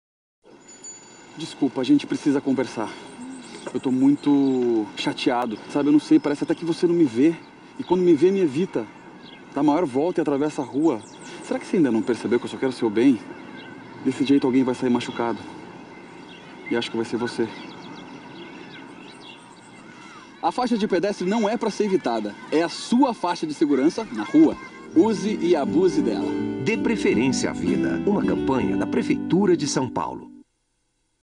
Relationship (portfolio - nova/sb - Campanha de Proteção aos Pedestres)
This film, continuation of the campaign, the crosswalkman discusses the relationship.